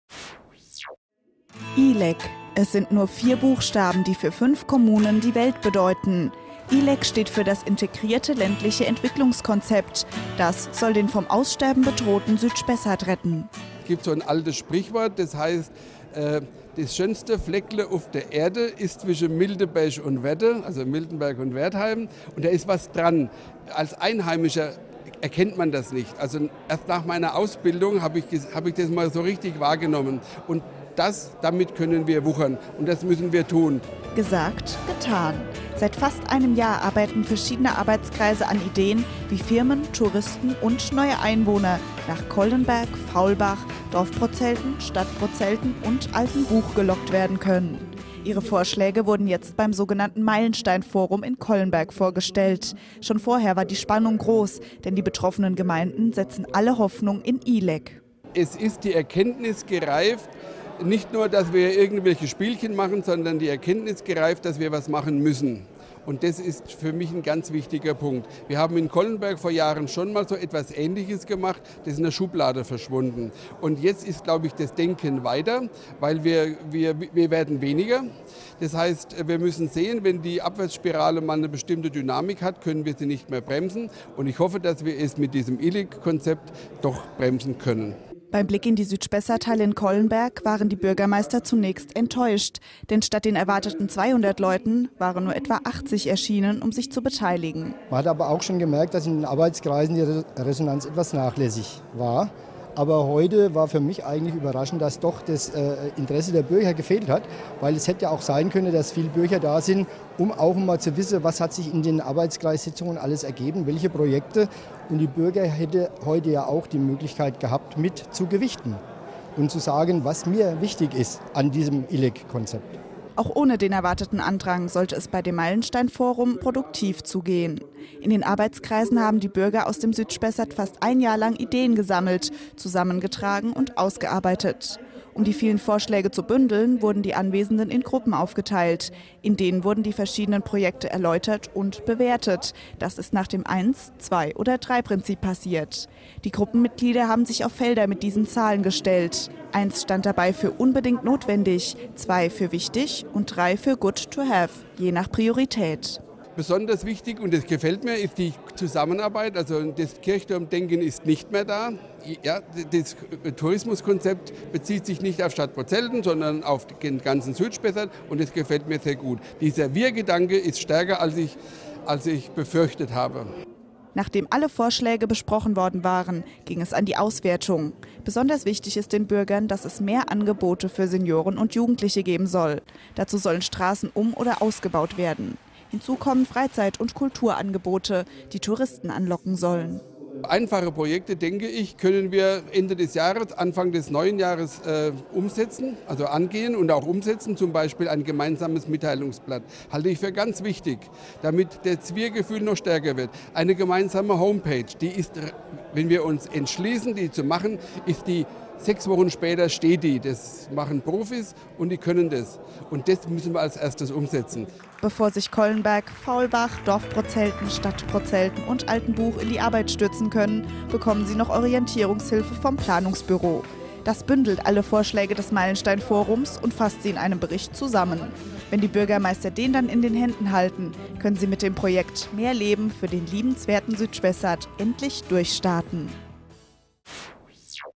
Collenberg: ILEK stellt Projekte zur Belebung Südspessart vor
Gähnende Leere im Südspessart - immer mehr Menschen und Firmen wandern ab. Dagegen wollen die Gemeinden dort vorgehen und haben ILEK ins Leben gerufen - das integrative ländliche Entwicklungskonzept. Jetzt hat dazu das so genannte Meilensteinforum stattgefunden, bei dem Projekte vorgeschlagen wurden.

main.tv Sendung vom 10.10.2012